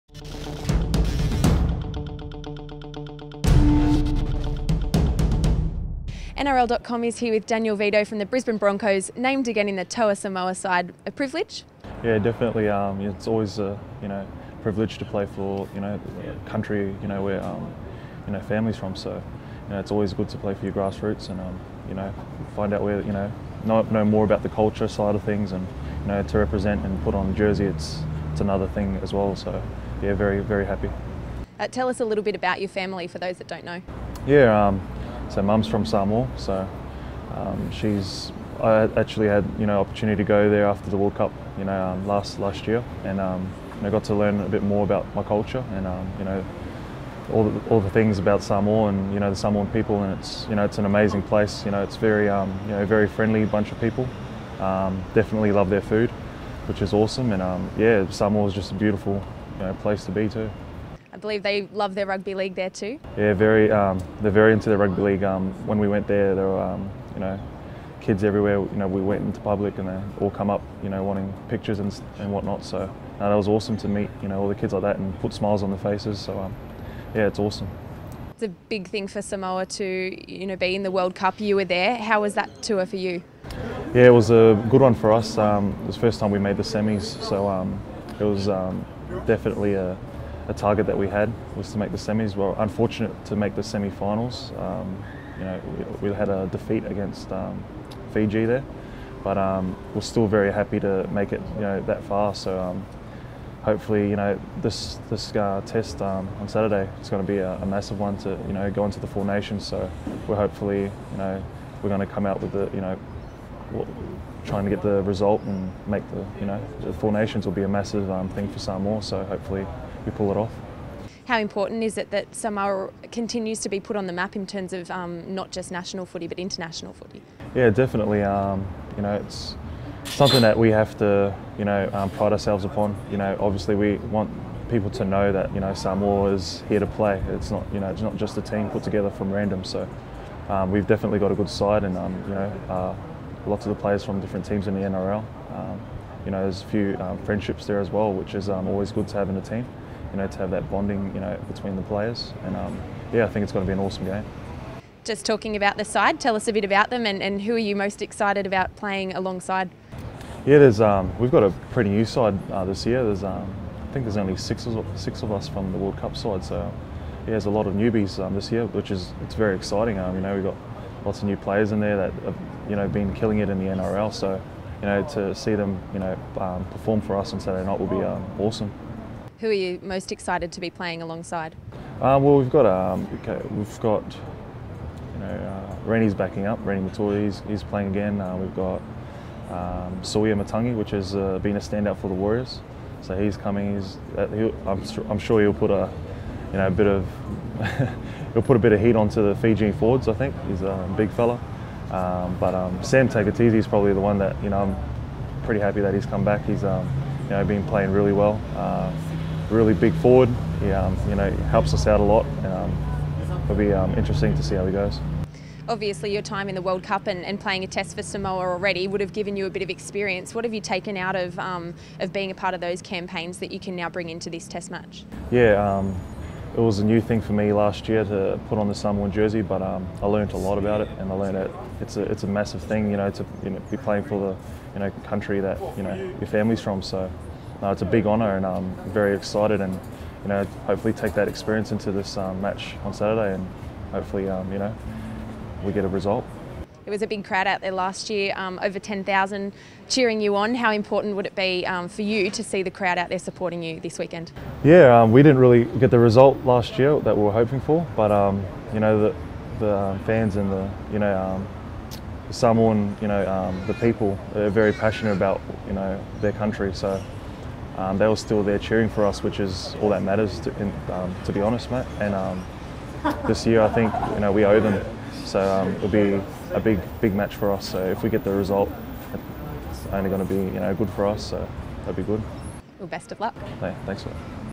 Vidot excited to play for Samoa
Daniel Vidot says he can't wait to put on the Samoa jersey and play against Fiji for a Four Nations spot at the end of the year